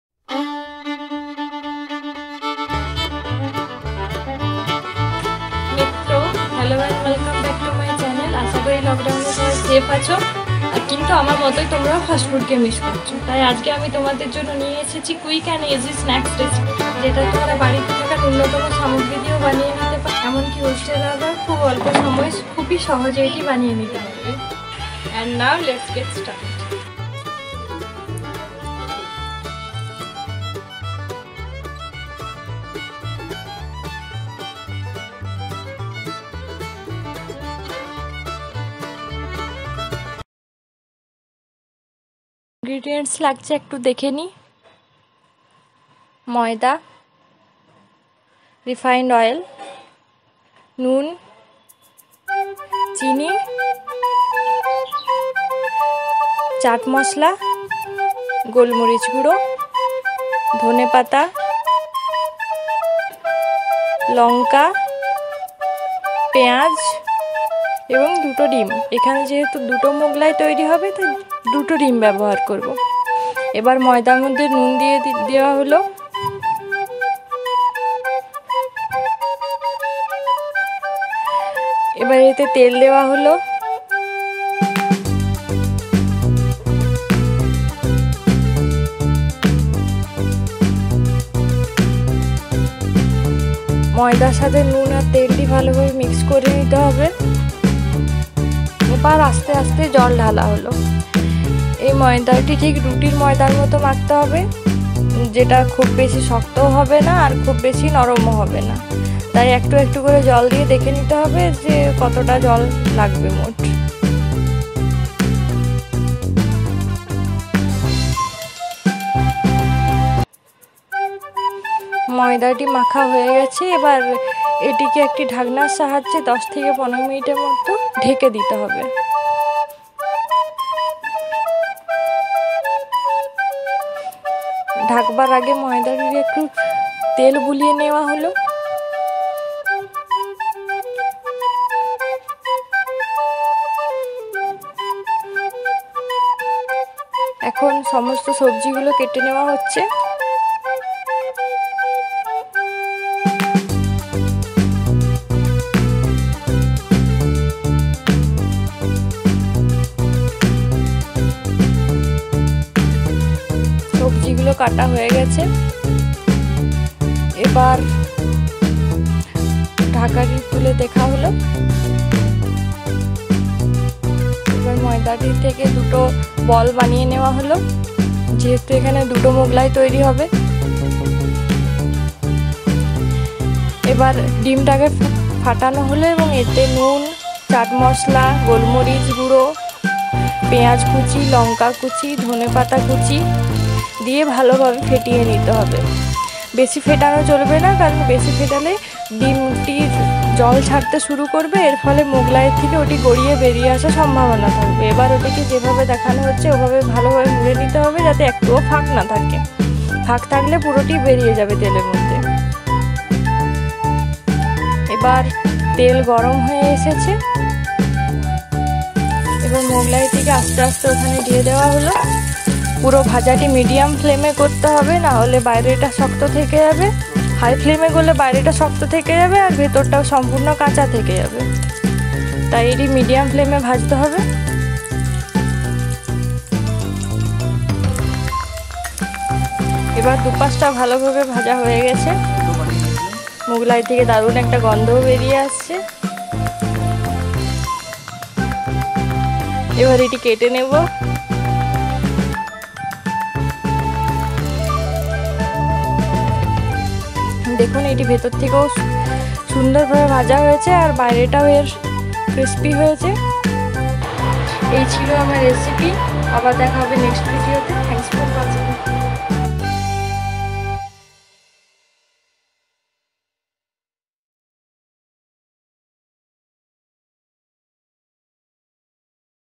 II HOSTELER STYLE MUGHLAI WITH MINIMAL INGREDIENTS II SOME MINUTES WITH KAMALIKA
Ei Lockdown e ar bairer Fastfood ke miss kora noy, baritei baniye phelun resturant er moto mughlai tao abar minimal ingredients use kore...
video to sesh porjonto dekhun ar valo lagle video ti ke like, Friends der sathe Share ar aro sohoj sohoj recipe pete amar channel ti ke subscribe korun, and hit the bell icon for instant notification.